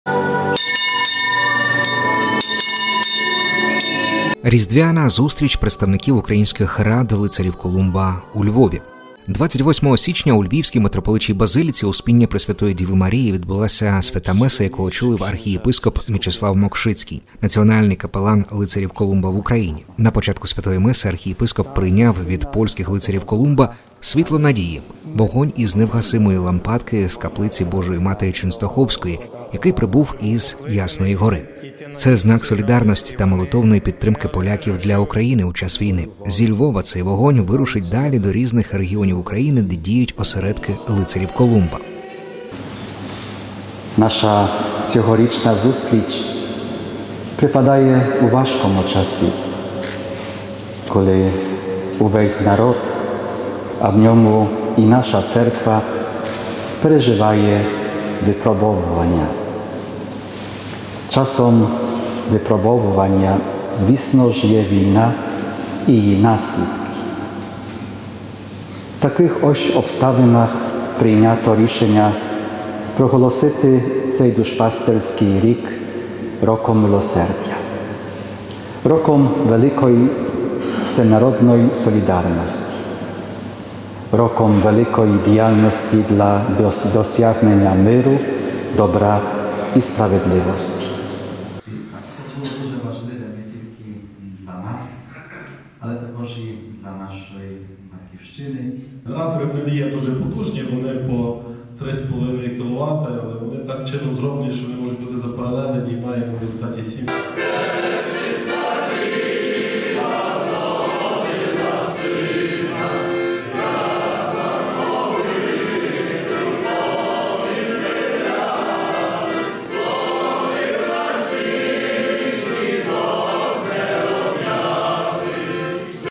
Різдвяна зустріч представників українських Рад Лицарів Колумба у Львові.
28 січня, в суботу, у Львівській Митрополичій базиліці Успіння Пресвятої Діви Марії відбулася щорічна Різдвяна зустріч представників українських Рад Лицарів Колумба.
Святу Месу, у співслужінні з капеланами Рад, очолив Національний Капелан Лицарів Колумба в Україні архієпископ Мечислав Мокшицький, митрополит Львівський.
На початку Святої Меси Митрополит прийняв від польських Лицарів Колумба «Світло надії» - вогонь із вічної лампади в каплиці Божої Матері Ченстоховської, який прибув з Ясної Гори на знак солідарності та молитовної підтримки для України у часі війни та вирушить зі Львова у всі регіони України.
У своїй проповіді Архієпископ назвав милосердя тим вогнем, який треба передавати світові та звернув увагу всіх присутніх на те, що проголошений єпископами України Рік Милосердя має бути часом великої всенародної солідарності, великої діяльності для досягнення миру, добра і справедливості:
-Будучи спільнотою Церкви, хочемо формувати нашу діяльність відповідно до думок і заповідей Бога, шляхом милосердя, на якому видно людей, які потребують допомоги. Робимо це, адже хочемо, як говорив св. Йоан Павло ІІ, у Божому Милосерді вбачати порятунок людства і щастя кожної людини зокрема. Тому теж із св. Йоаном Павлом ІІ закликаємо, будьмо апостолами Милосердя, бо воно є вогнем, який треба передавати теперішньому світу. (…) А світло з Ясної Гори довіряю усім, хто прагне його прийняти. Довіряю його і вам, Лицарі Колумба, щоб ви першими вирушили в путь із цим світлом миру і занесли його людям обтяженим війною. В ім’я переможної Марії, нехай цей пломінь допоможе нам потоптати зло і відкинути його від меж нашого дому.
Після завершення Святої Меси відбулися підсумково-організаційні збори, на яких відзвітували за рік, що минув і накреслили плани на рік, який розпочавсята, а також спільне колядування у митрополичій курії Львівської архідієцезії.